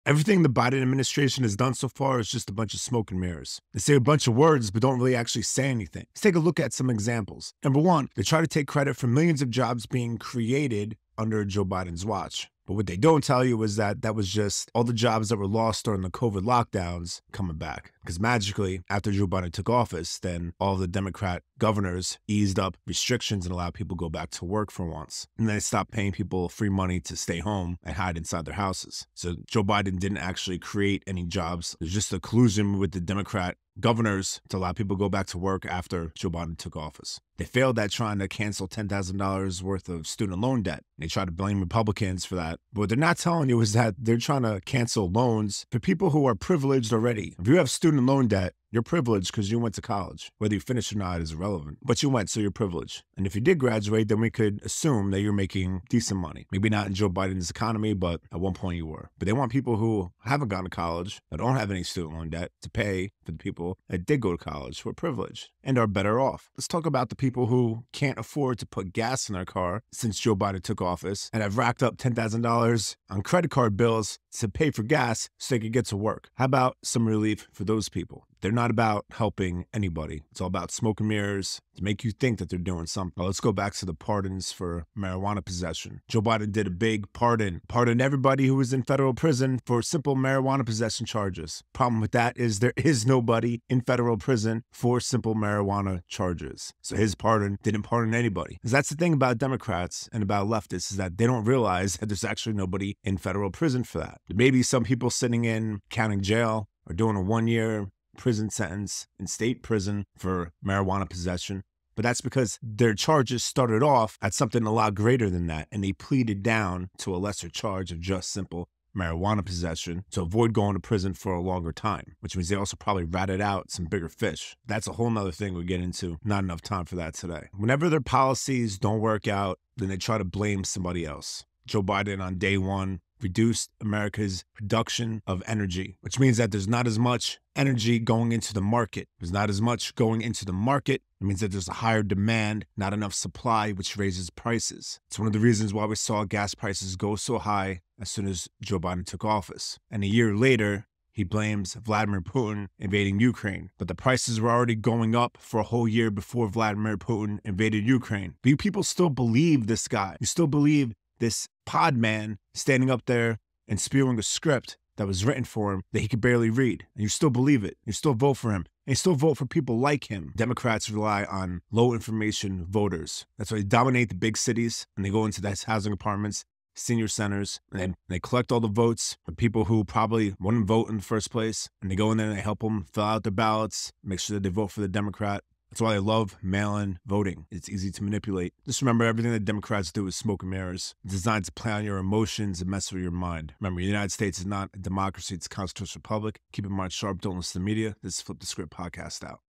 BREAKING: Joe Biden and the democrats fake accomplishments
Sometimes its easy for a politician to spin their own fake history and try to convince you that its true. After all, they control the history books. They control the media. They control most of the media in this country. So when it comes to accomplishments of democrats, what we read in history books and what we hear on cable news isn't always accurate. In this video, I break down a few of the Biden Administration "accomplishments" that are either misleading or straight up fake.